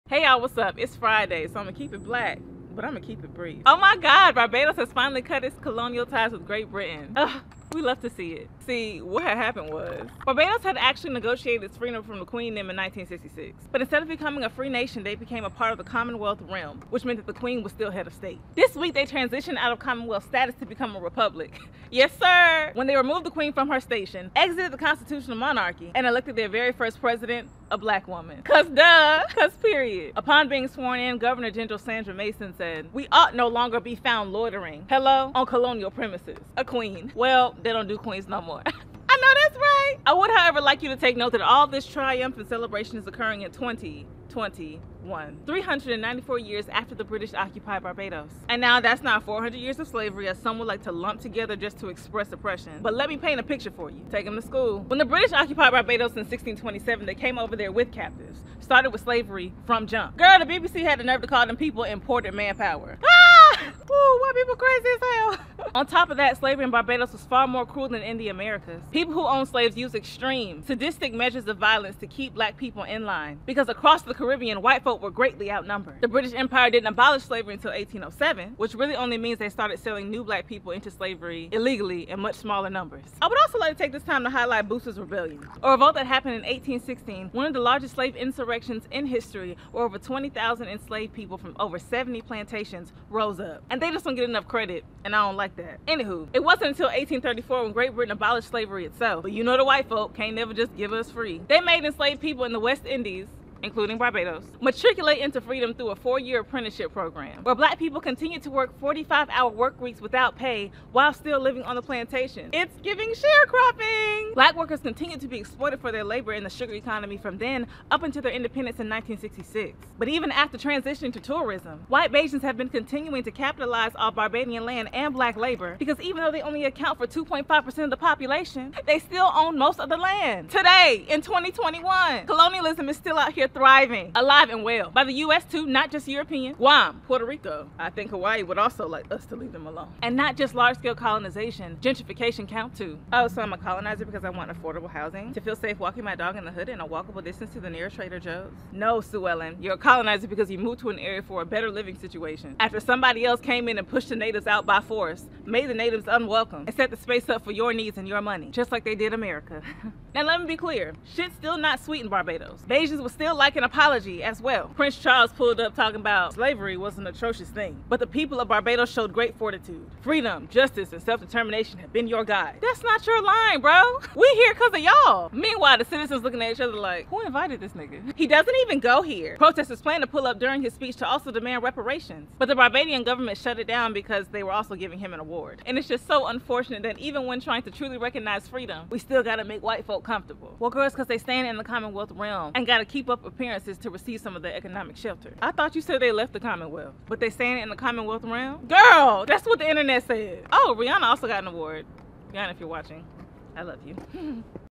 Parking Lot Pimpin| On Barbados Becoming a Republic
This week on PLP, Lynae discusses Barbados recent separation from Great Britain as they become a republic, and educates viewers on the history of the relationship between the two.